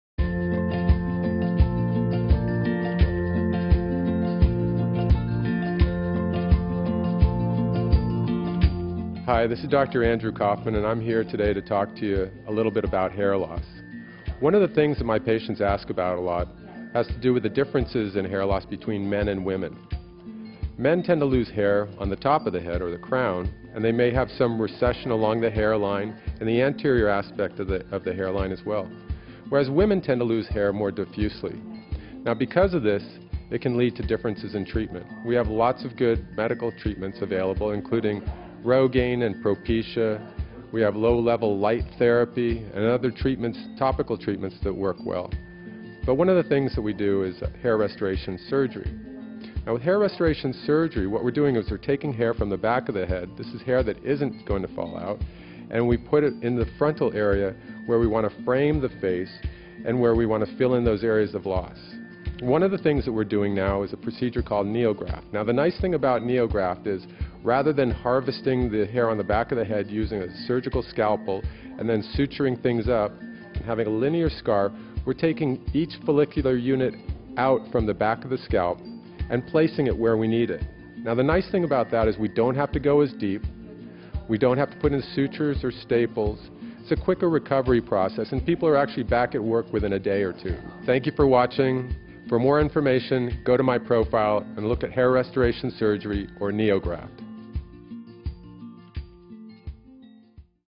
Men and Women Lose Hair Differently - Here's Why That Matters
Dr. Kaufman talks about hair loss in men and women and how they differ.

Offices in:
Thousand Oaks, CA
Santa Barbara, CA
Tustin, CA